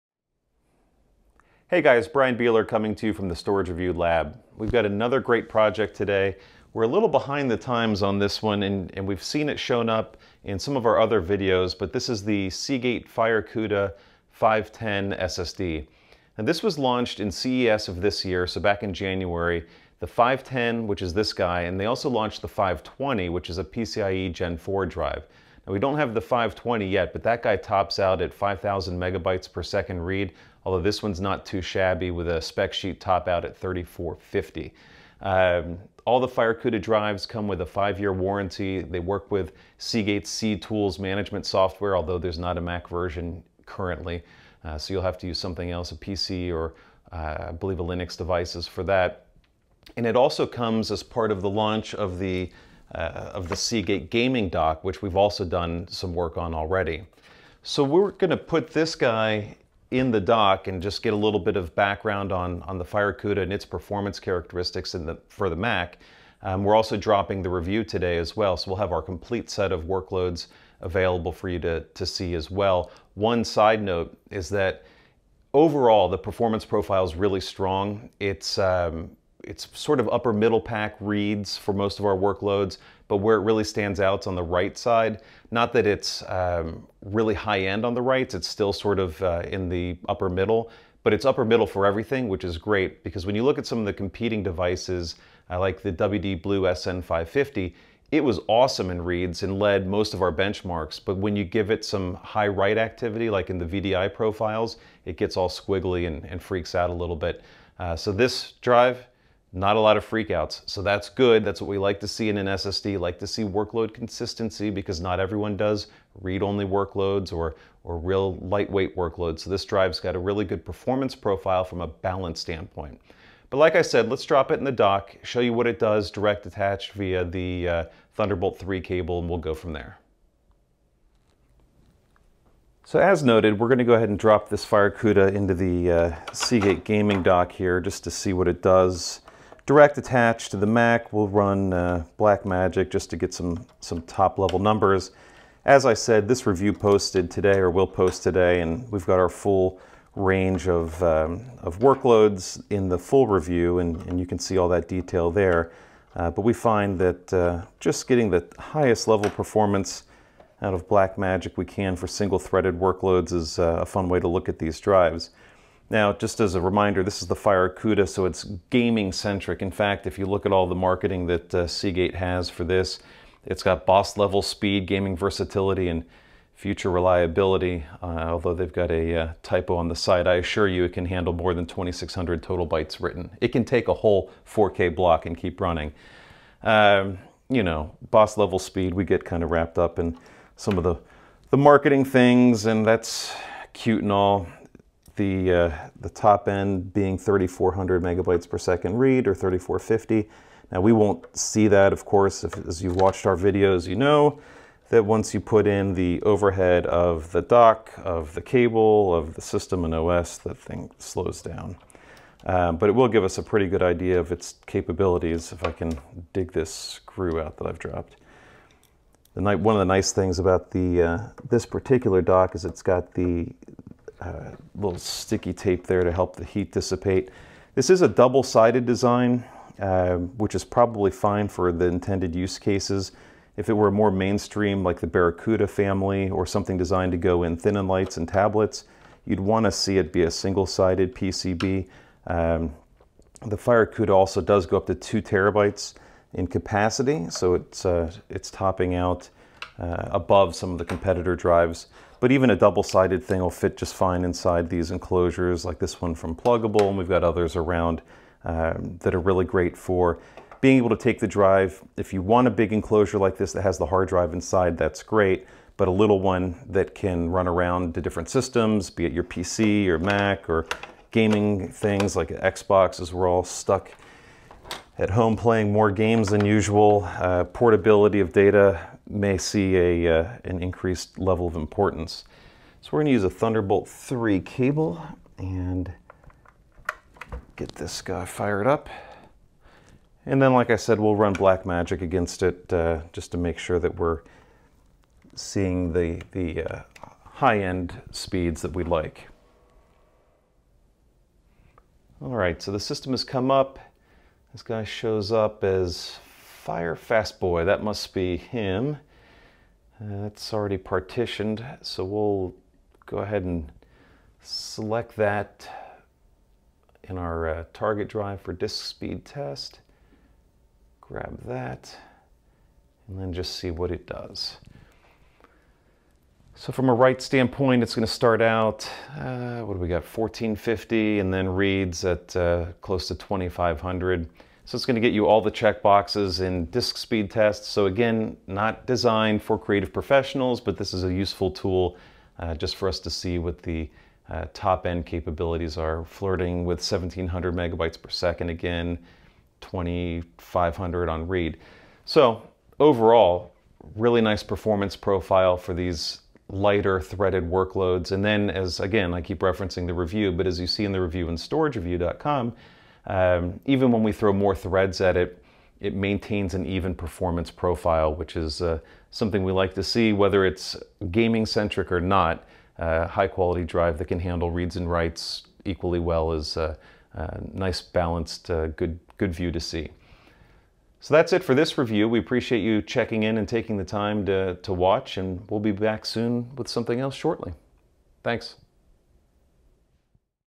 Seagate FireCuda 510 SSD Review
Seagate launched the FireCuda 510 SSD and 520 SSD at CES earlier this year, two entires in the gaming SSD space. Compatible with the Seagate gaming dock, the drives can be used in a wide variety of enclosures or within a PC to handle gaming-centric workloads. The FireCuda comes in capacities up to 2TB and in our testing shows very even read and write performance, which provides an even experience for the end user. In this video we highlight the overall performance from our review and connect it via the dock for a quick Black Magic test.